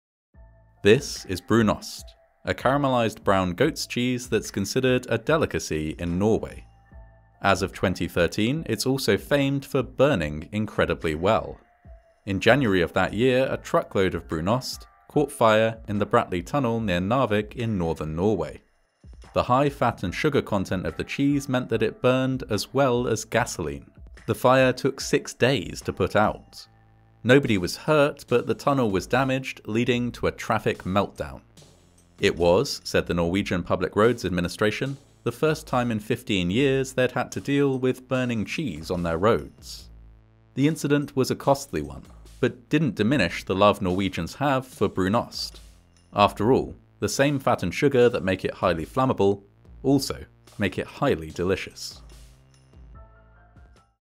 The Goat Cheese Fire of 2013 | Fascinating Horror Shorts
Brunost is a delicacy in Norway, but the caramelised cheese is, it turns out, also very flammable...